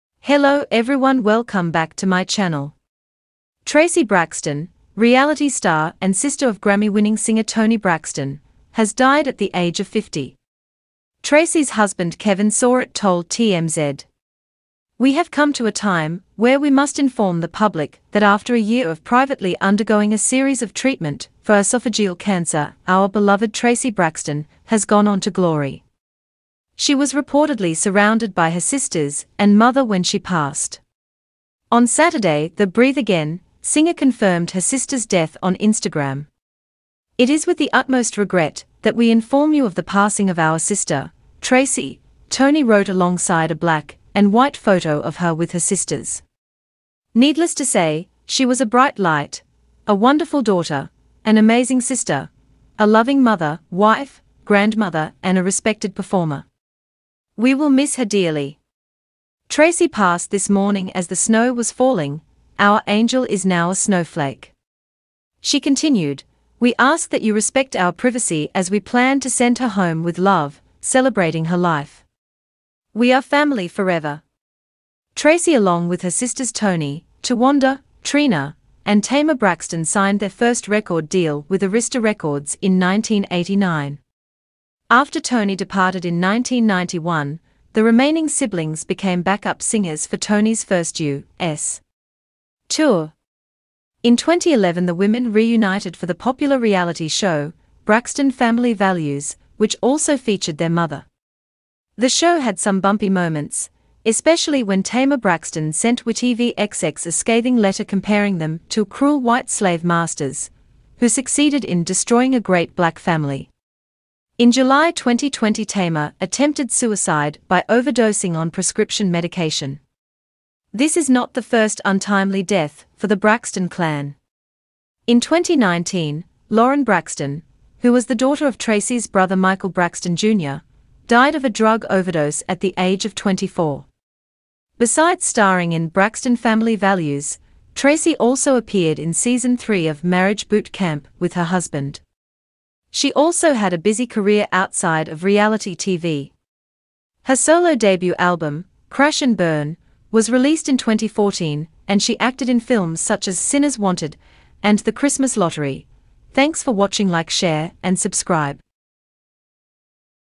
R.I.P! Its With Heavy Hearts To Report Death Of Traci Braxton Due To Painful Disease
Traci Braxton, reality star and sister of Grammy-winning singer Toni Braxton, has died at the age of 50.